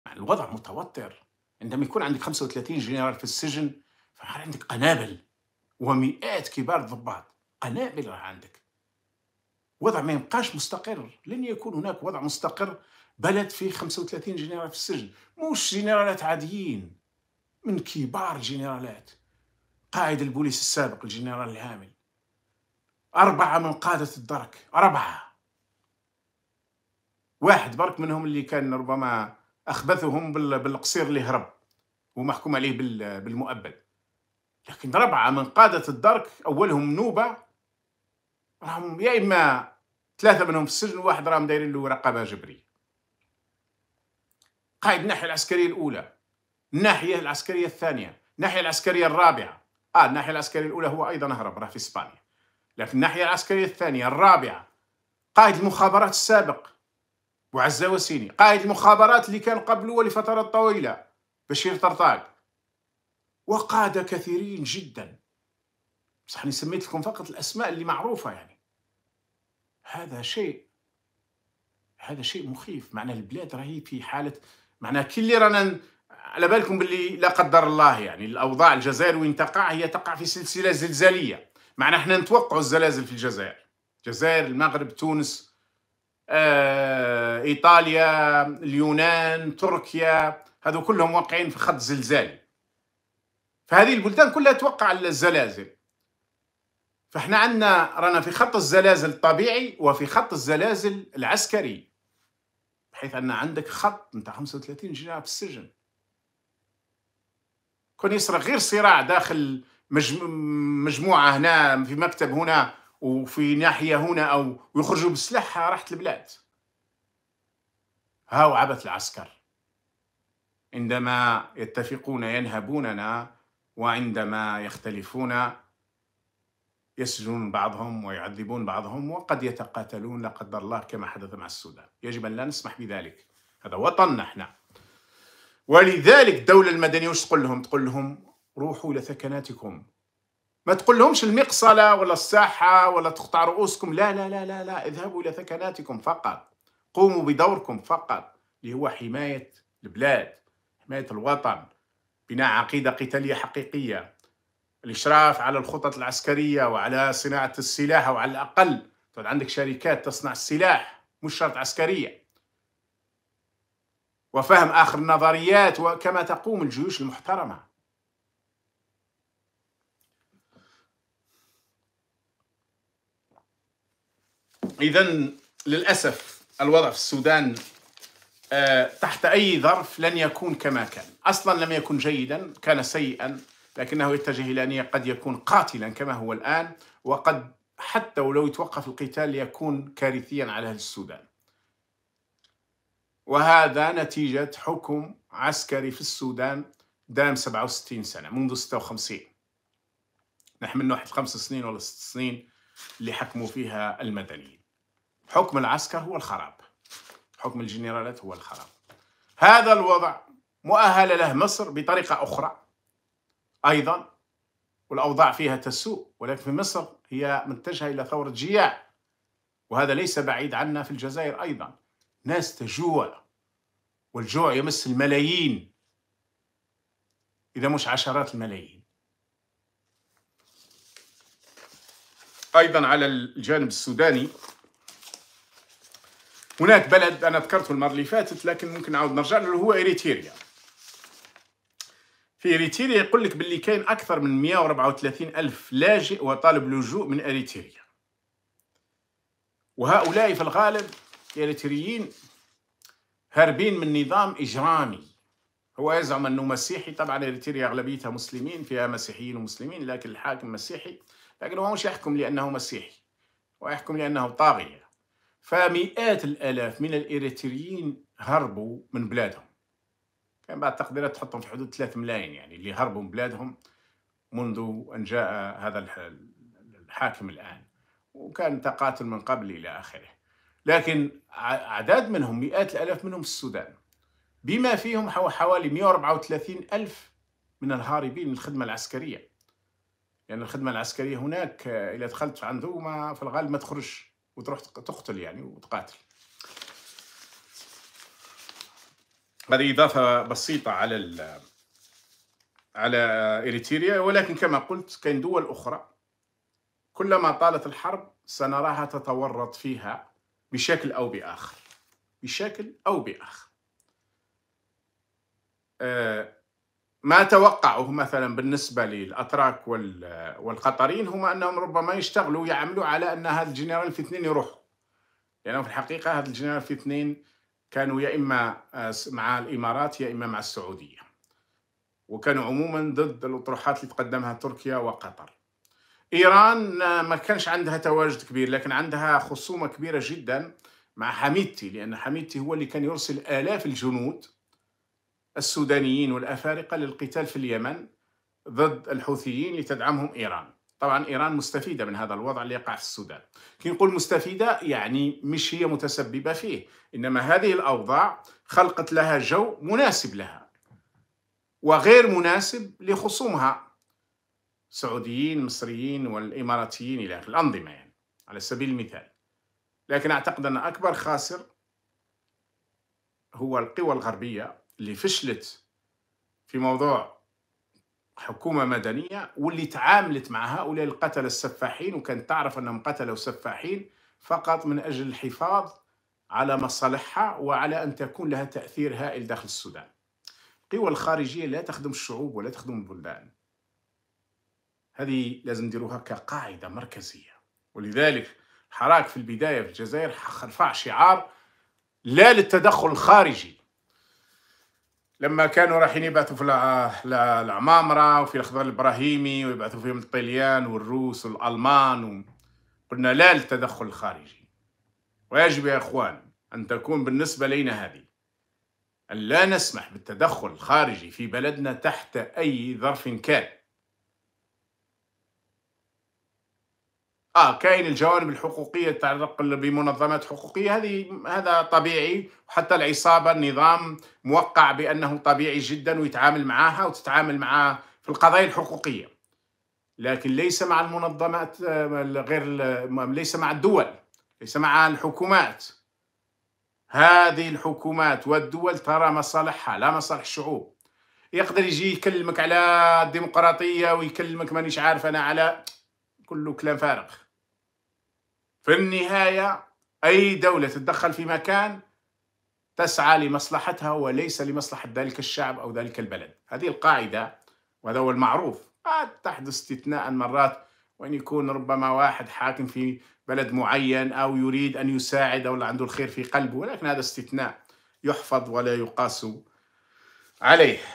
الوضع متوتر...35 جنرال في السجن...نحن في خط زلزال عسكري
محمد العربي زيتوت 22 أفريل 2023